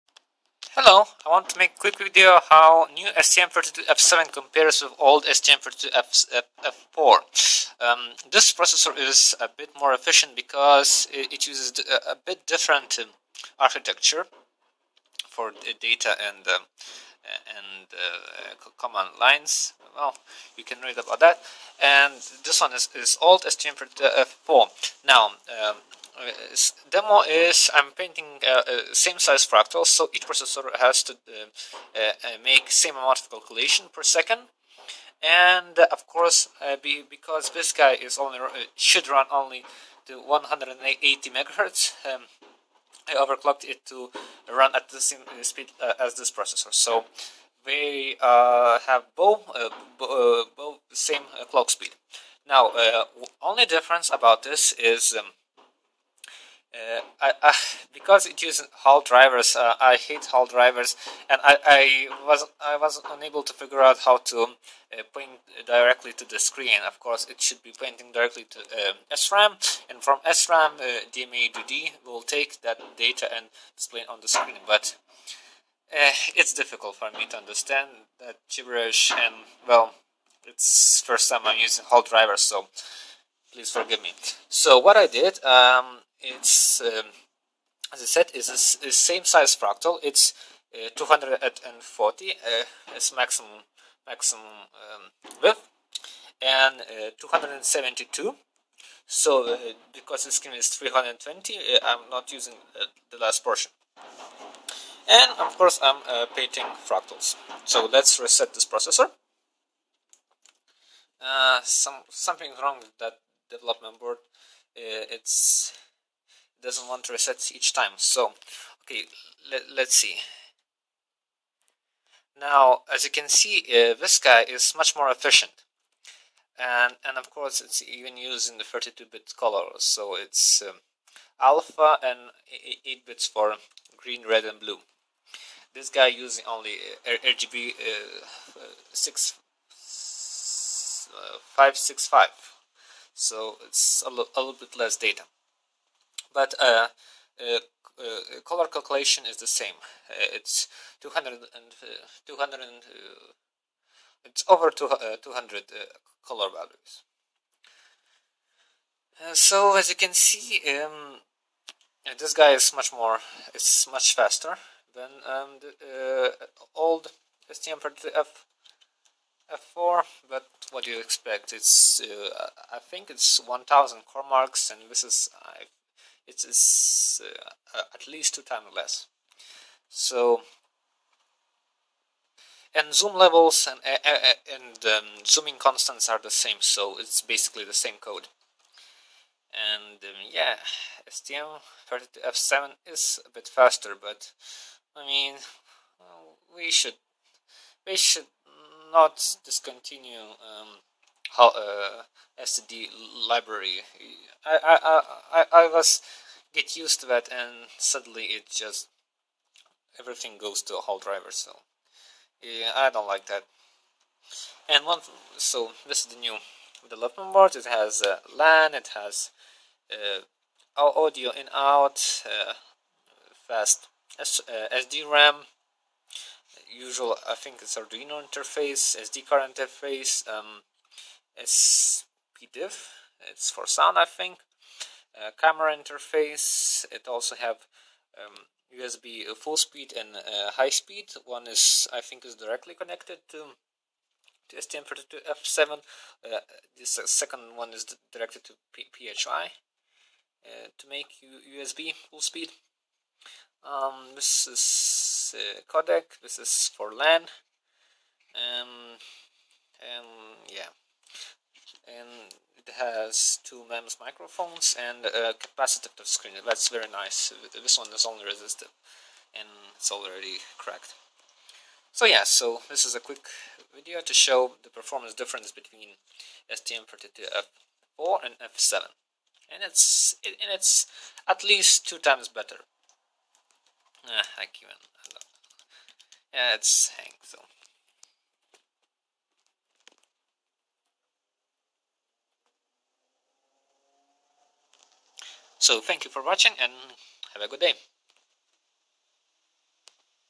STM32F7 vs STM32F4 in f32 Mandelbrot calculation
Simple video to show how much faster STM32F7 is against STM32F4 running at same clock speed ! This is because more efficient M7 core implementation